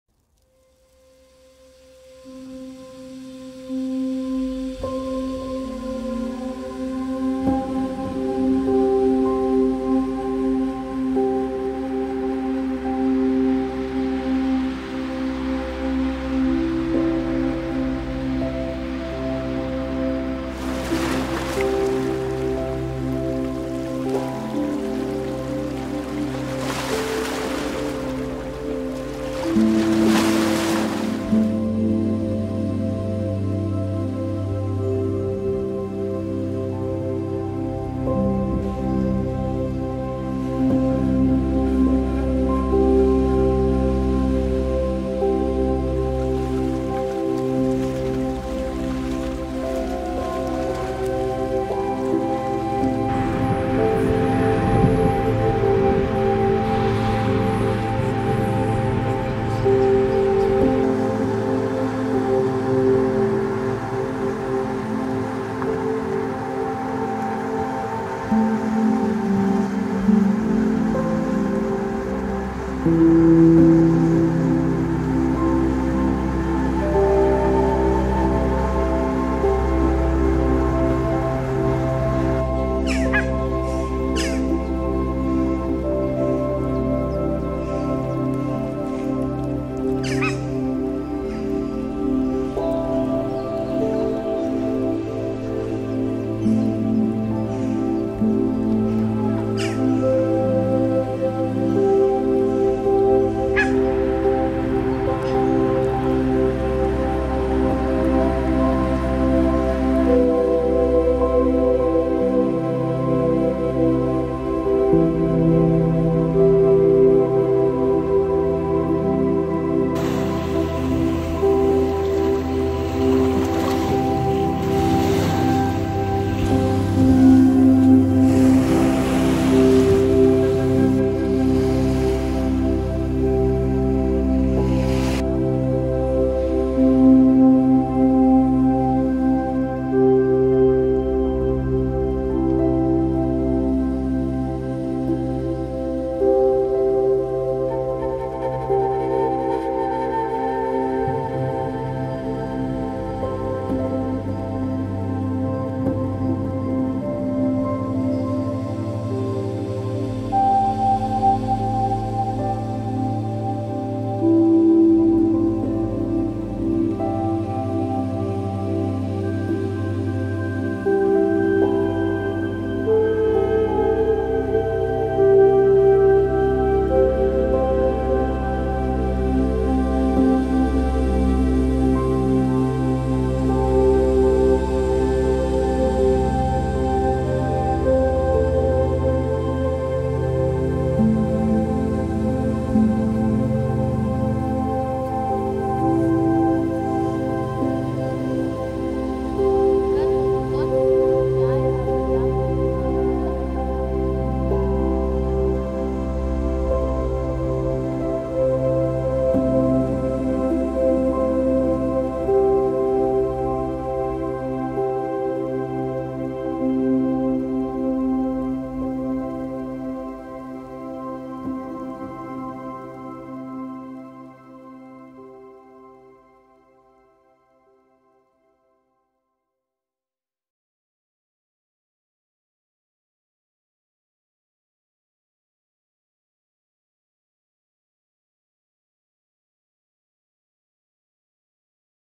Cinematic movie - Emotion.
This cinematic 4k video is filmed with a digital camera Sony Zve10 and a drone Dji Mini 3 Pro. In this movie, you can see stunning Scandinavian colorful landscapes and amazing danish coastal forests, nature, also a beautiful Nyborg city on the horizon. Relaxing music and Autumn colors.